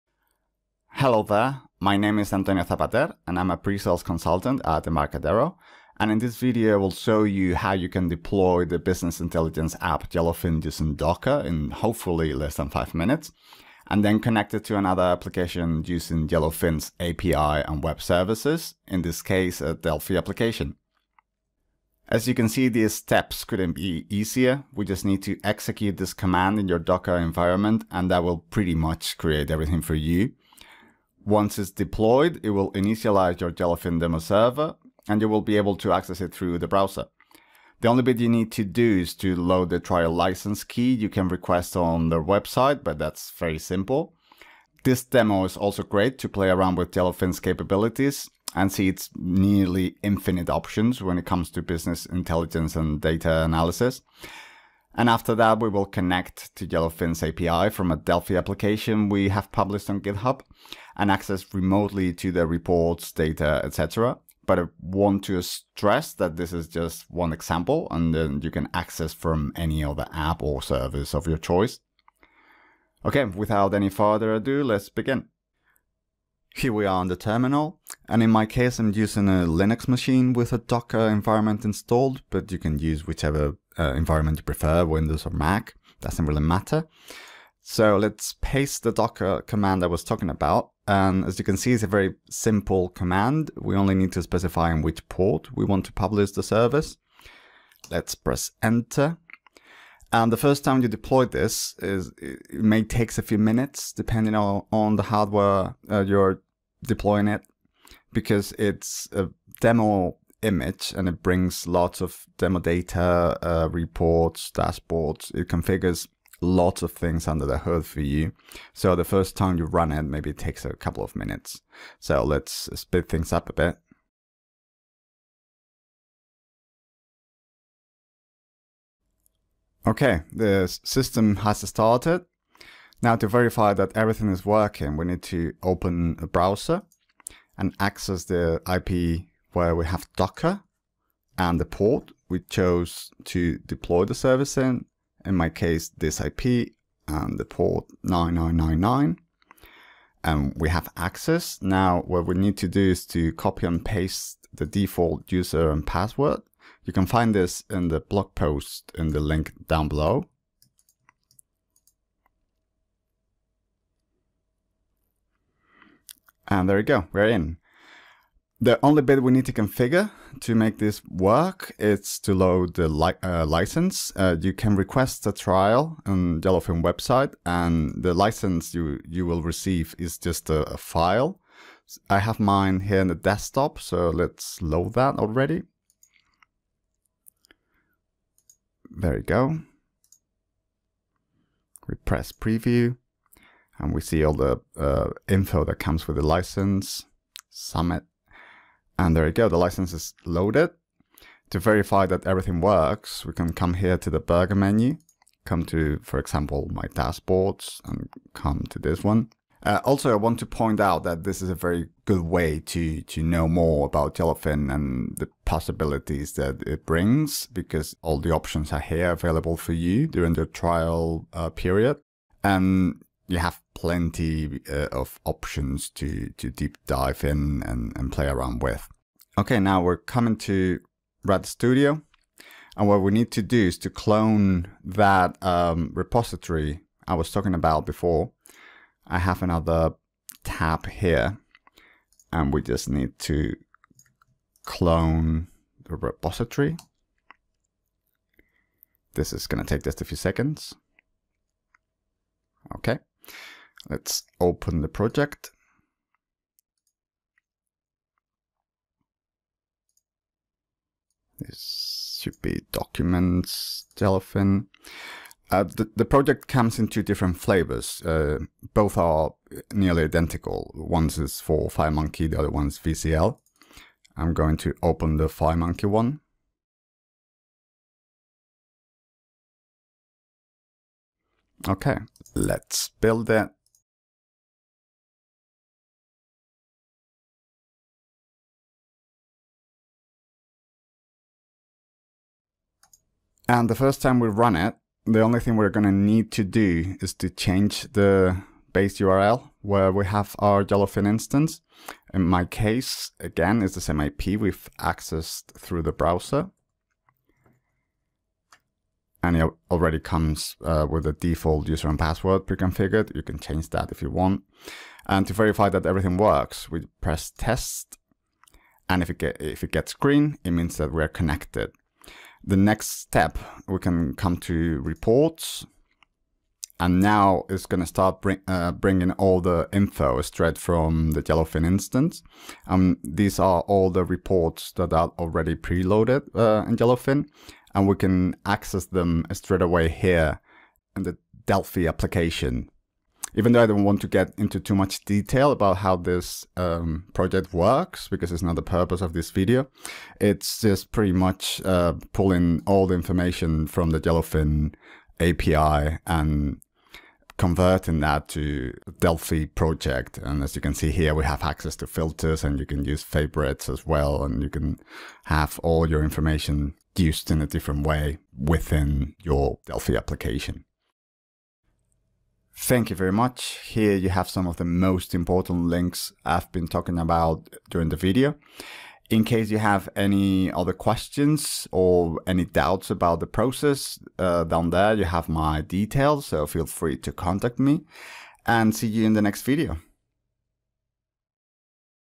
Deploying Yellowfin using Docker - And using it inside Delphi Apps in less than 5 minutes!
The Docker image for Yellowfin enables developers to get started with sample data and configurations. This video shows how to download and set up the docker image, including loading in your developer/trial license. Once Yellowfin is running you can then use the Delphi Example from Github to see it working inside Delphi!